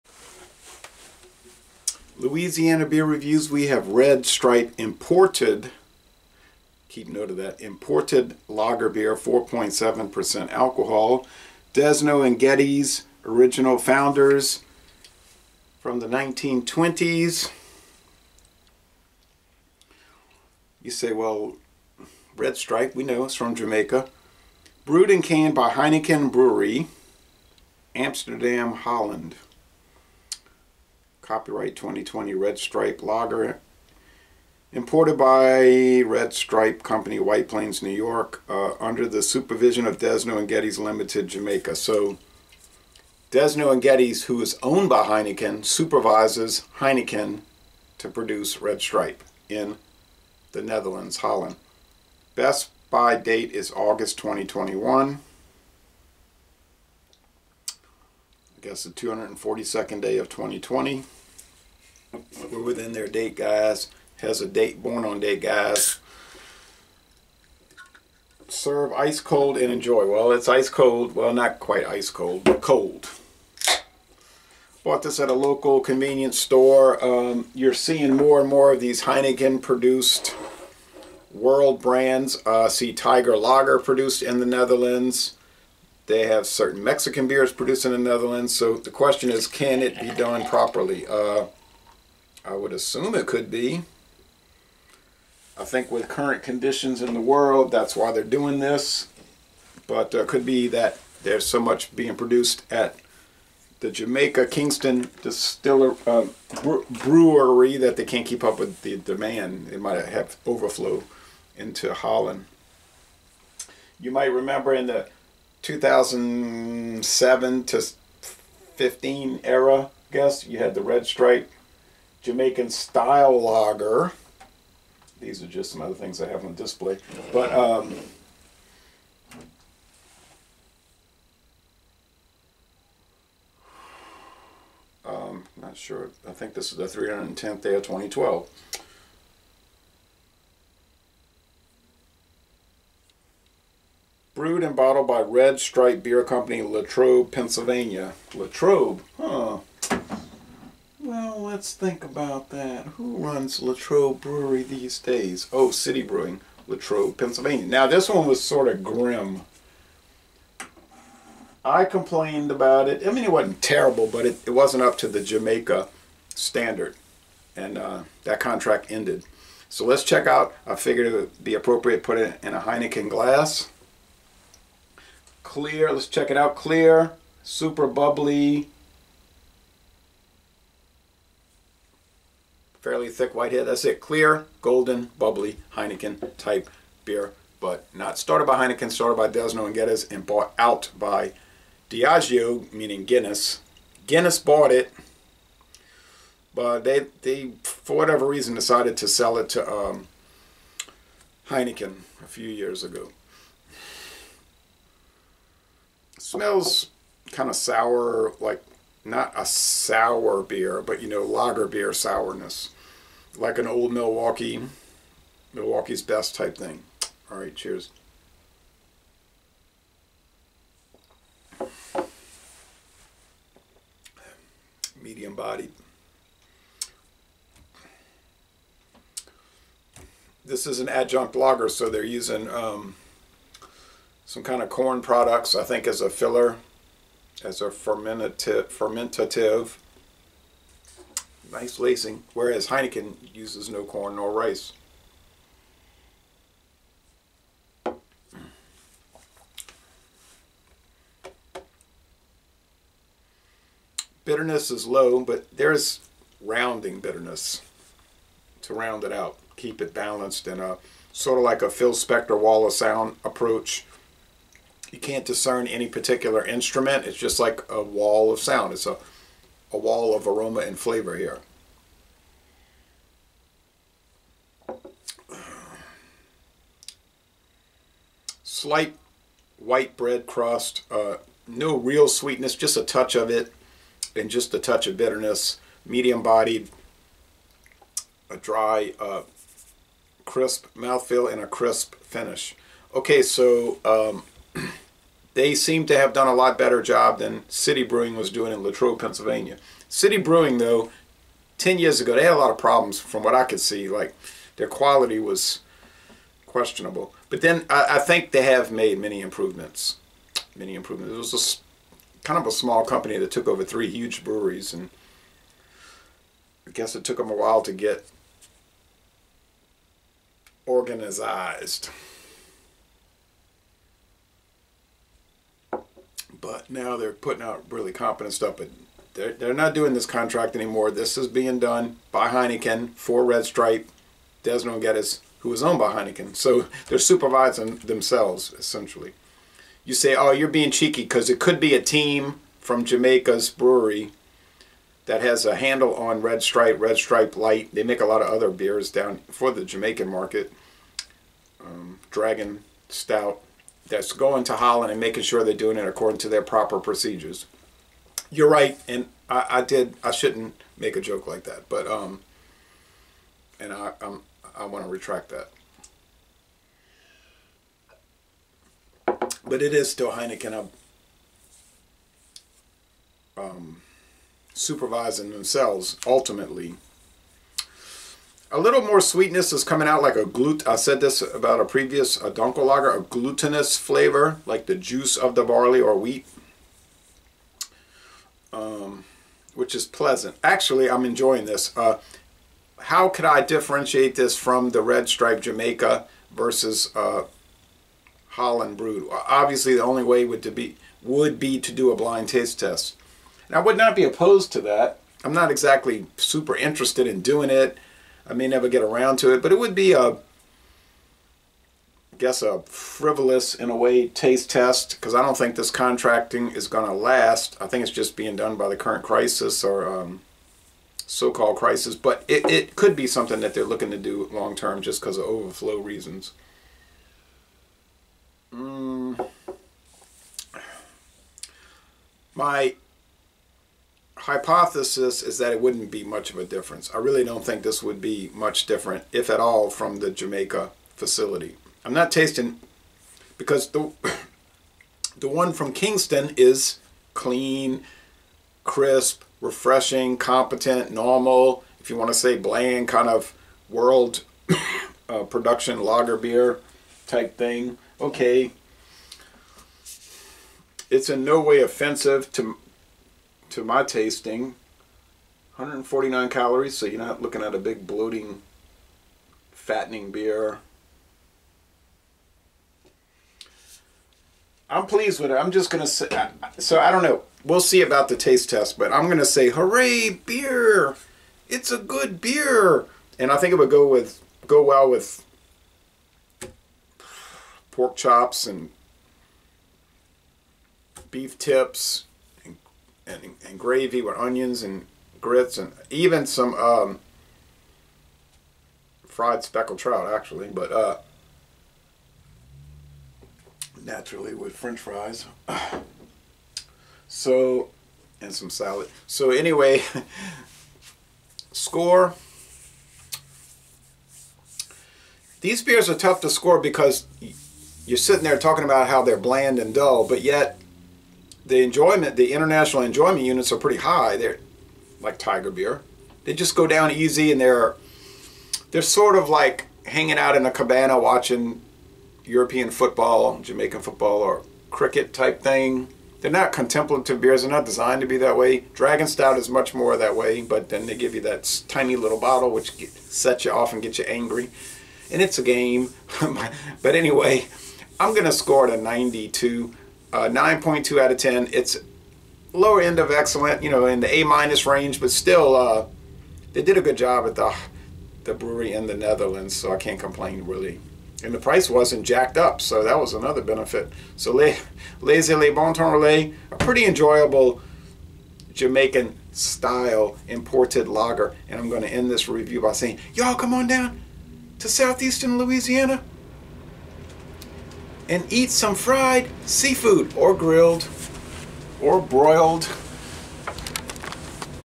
Louisiana Beer Reviews: Red Stripe Lager (Dutch-brewed version)
4.7% alcohol. Introduced in 1928. Brewed in The Netherlands by Heineken.

"A beautiful amber lager with low butterscotch flavour, full bodied and a smooth post palate sensation. Lager beer, light gold colour."